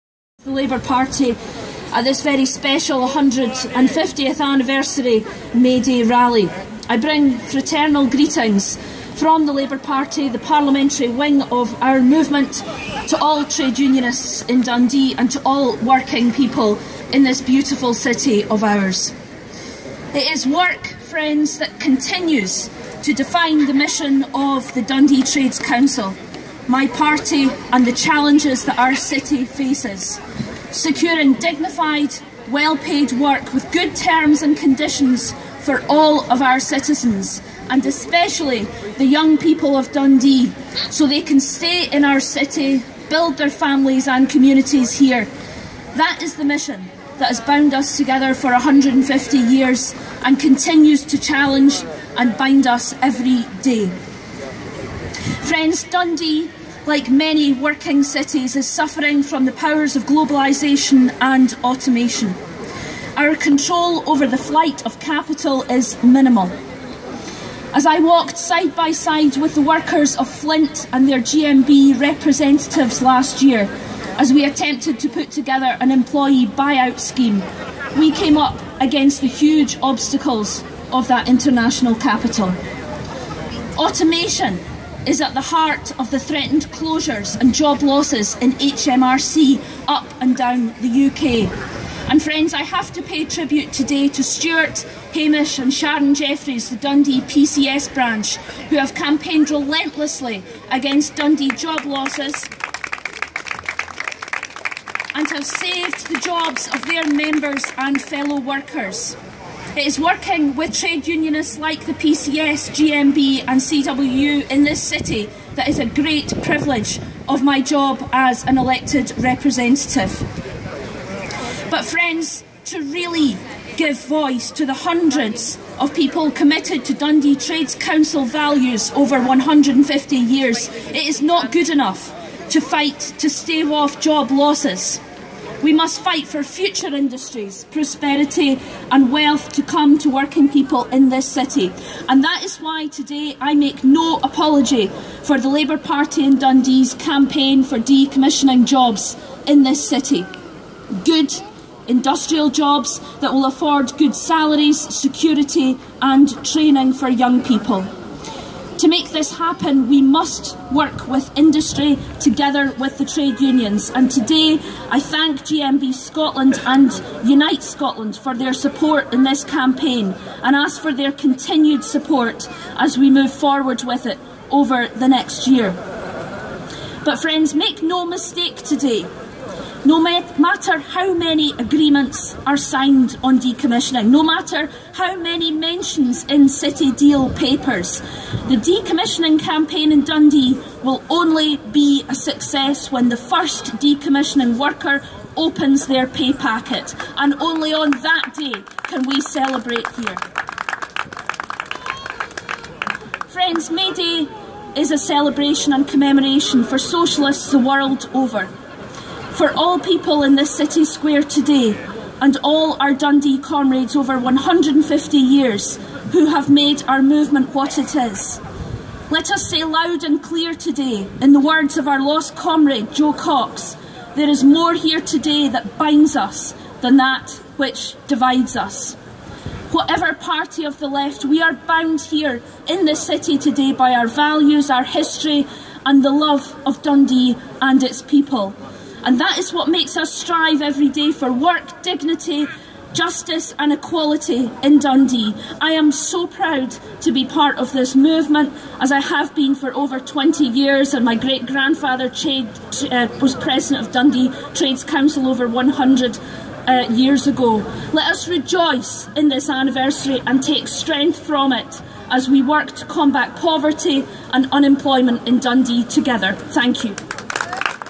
Jenny Marra MSP at Dundee Mayday Rally
Jenny was one of the speakers at the 2017 Dundee Mayday Rally.